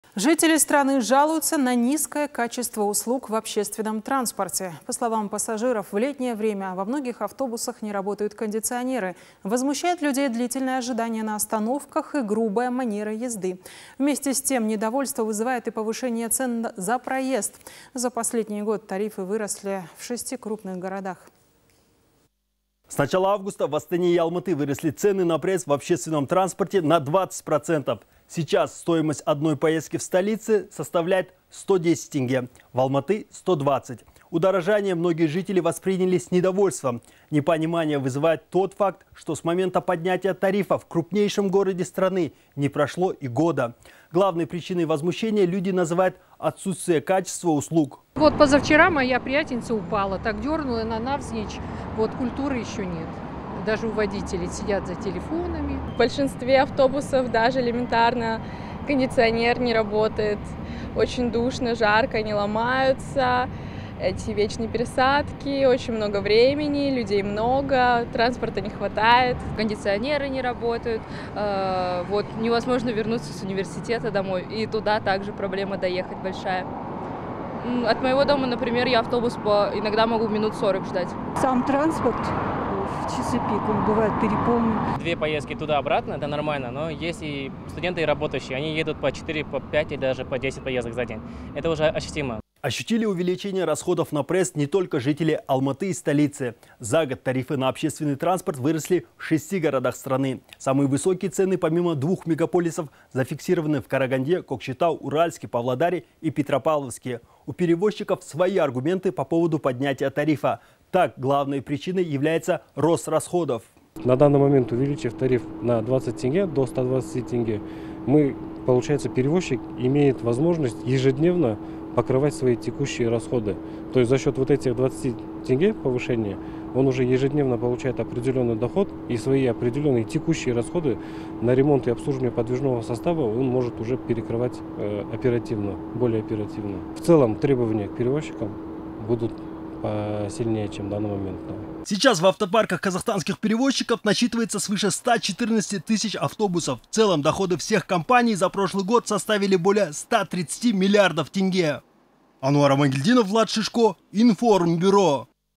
Почему в общественном транспорте растут тарифы при низком качестве услуг?
Жители страны жалуются на низкое качество услуг в общественном транспорте. По словам пассажиров, в летнее время во многих автобусах не работают кондиционеры. Возмущает людей длительное ожидание на остановках и грубая манера езды.  Вместе с тем, недовольство взывает и повышение цен на проезд. За последний год тарифы выросли в шести крупных городах.
АНУАР АМАНГЕЛЬДИНОВ, корреспондент:
- С начала августа в Астане и Алматы выросли цены на проезд в общественном транспорте на 20 процентов. Сейчас стоимость одной поездки в столице составляет - сто десять тенге, в Алматы – сто двадцать. Удорожание многие жители восприняли с недовольством. Непонимание вызывает тот факт, что с момента поднятия тарифа в мегаполисе не прошло и года. Главной причиной возмущения люди называют отсутствия качества услуг.
- Вот позавчера моя приятельница упала. Так дернуло. Вот культуры еще нет, даже у водителей, сидят за телефонами.
- В большинстве автобусов даже элементарно кондиционеры не работают. Очень душно, жарко, они ломаются. Эти вечные пересадки. Очень много времени. Людей много, транспорта не хватает.
- Кондиционеры не работают. Невозможно вернуться с университета домой и туда также проблема доехать большая. От моего дома например автобус могу по минут 40 ждать.
- Сам транспорт в часы-пик переполнен.
- 2 поездки, туда обратно это нормально. Но если студенты и работающие они едут по 4,5 и даже 10 поездок за день. Это уже ощутимо.
АНУАР АМАНГЕЛЬДИНОВ, корреспондент:
- Ощутили увеличение расходов на проезд не только жители Алматы и столицы. За год тарифы на общественный транспорт выросли в шести городах страны. Самые высокие цены помимо двух мегаполисов зафиксированы в Караганде, Кокшетау, Уральске, Павлодаре и Петропавловске. У перевозчиков свои аргументы по поводу поднятия тарифа.  Так, главной причиной является рост расходов.
БАУЫРЖАН КУМАРГАЗИН, заметситель руководителя управления городской мобильности:
- На данный момент увеличив тариф на 20 тенге до 120 тенге, получается перевозчик имеет возможность ежедневно покрывать свои текущие расходы. То есть за счет вот этих 20 тенге повышения он ежедневно получает доход и свои определенные текущие расходы на ремонт и обслуживание подвижного состава он уже может перекрывать оперативно. В целом требования к перевозчикам будут посильнее чем в данный момент.
Сейчас в автопарках казахстанских перевозчиков насчитывается свыше ста четырнадцати тысяч автобусов. В целом доходы всех компаний за прошлый год составили более ста тридцати миллиардов тенге. Ануар Амангельдинов, Влад Шишко, информбюро.